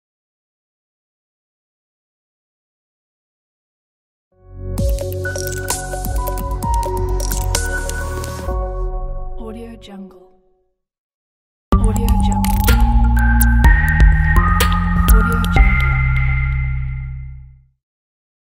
Colorful Logo | Free After Effects Templates
Project features:
1080p (1920×1080), 2K and 4K (3840×2160) resolutions
2 Logo Reveals
Compatible with CS6 and above
Easy color change
Universal Expressions
Free font used (link in help file)

TO MAC USERS: If password doesn't work, use this archive program
and extract password protected files without error.